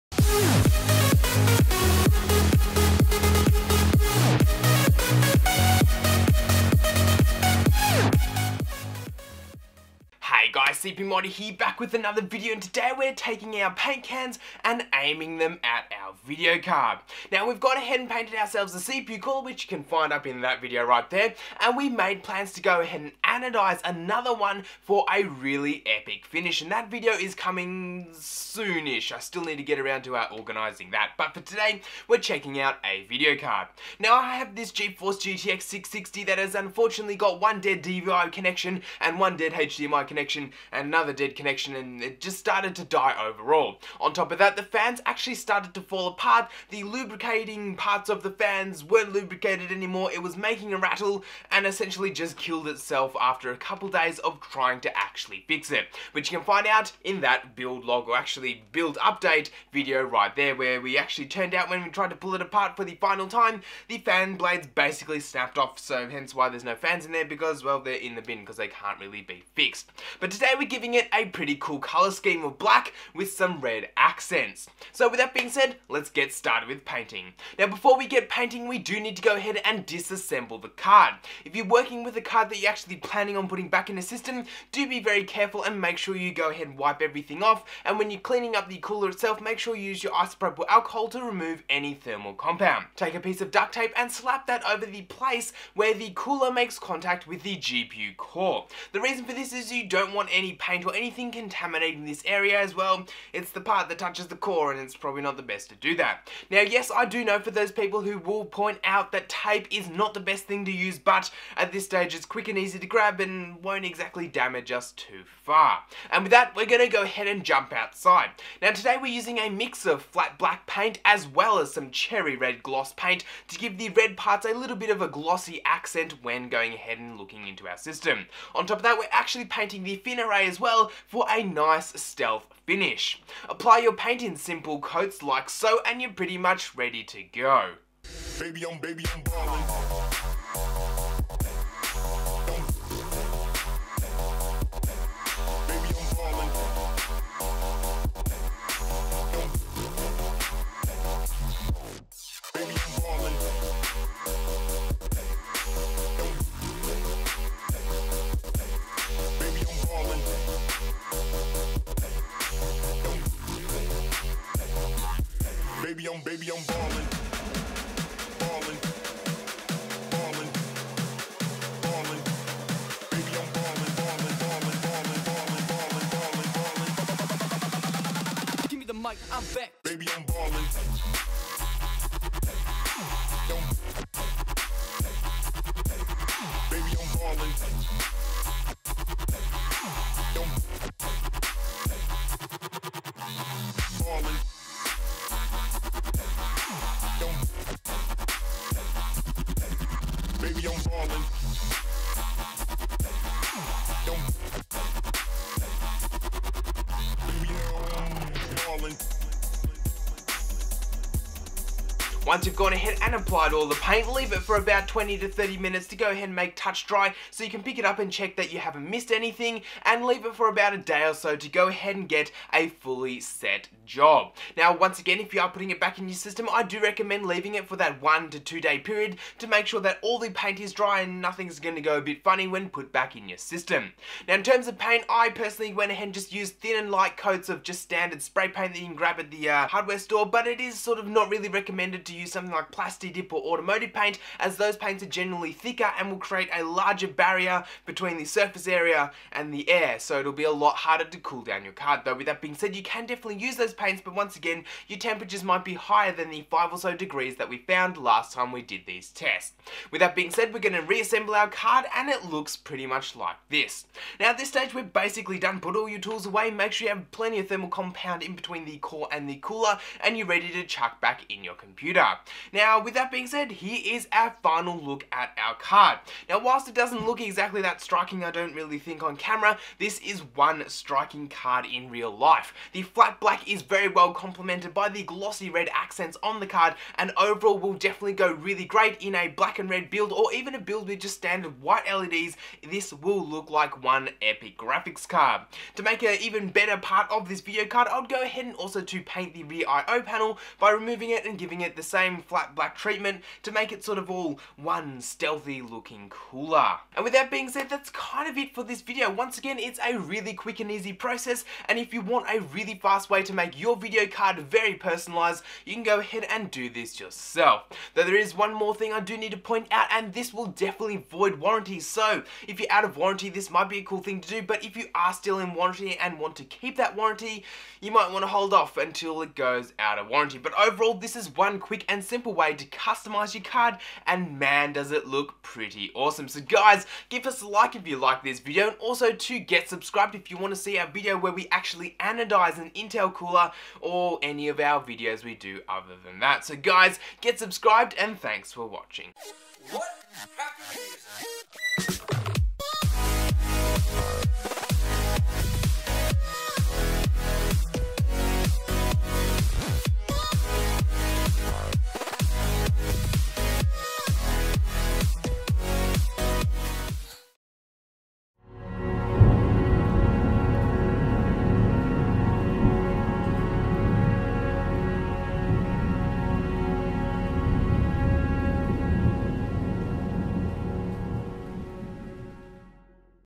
How To Paint a GPU
We have painted a cpu cooler so today we look at a gpu and how to paint it

Check out CPUmodder.com for more

      bit.do/cpumodder or use offer code CURIOUSTEACHER20
      to save

Intro inspiration: Official™ Designs, recreated in after effects